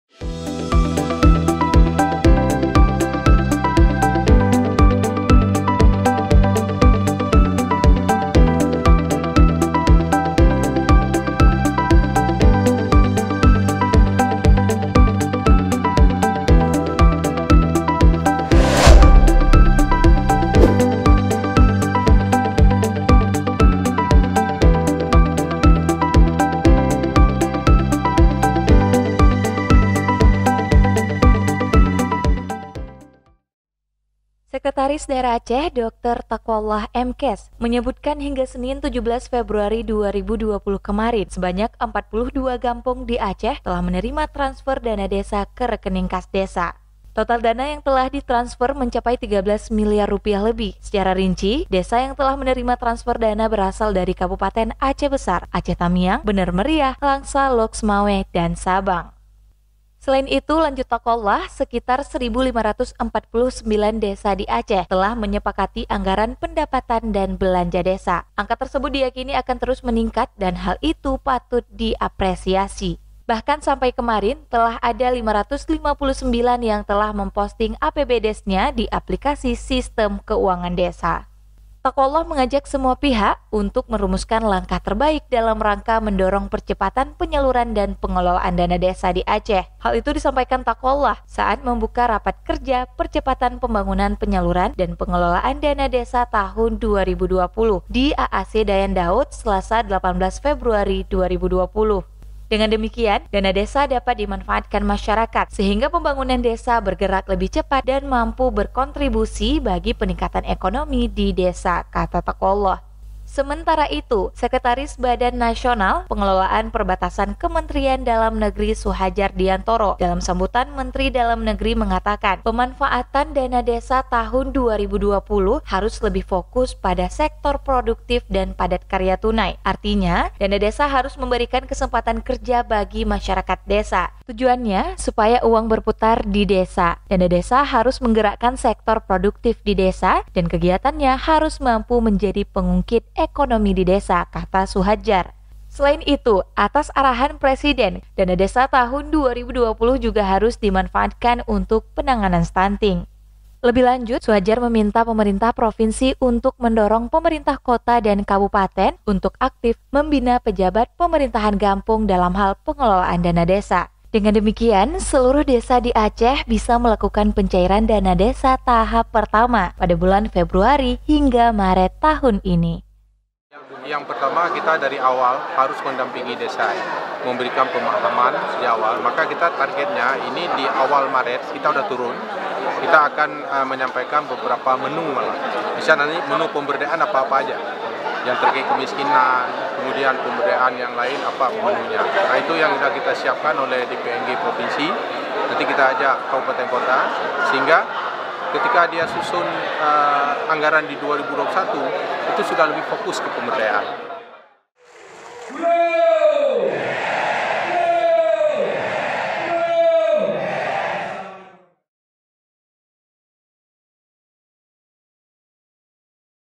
42 Gampong di Aceh Telah Terima Transfer Dana Desa
Banda Aceh - Sekretaris Daerah Aceh, dr. Taqwallah M.Kes., menyebutkan hingga Senin (17/02) kemarin, 42 gampong di Aceh telah menerima transfer dana desa ke Rekening Kas Desa (RKD). Total dana yang telah ditransfer mencapai Rp.13 miliar lebih. Secara rinci, desa yang telah menerima transfer dana berasal dari kabupaten Aceh Besar (15), Aceh Tamiang (9), Bener Meriah (9), Langsa (4), Lhokseumawe (3), Sabang (1).

Selain itu, lanjut Taqwallah, sekitar 1.549 desa/gampong di Aceh telah menyepakati Anggaran Pendapatan dan Belanja Desa (APBDes). Angka tersebut diyakini akan terus meningkat. Hal itu patut diapresiasi. Bahkan sampai kemarin, telah ada 559 yang telah memposting APBDes-nya di aplikasi Sistem Keuangan Desa (Siskeudes).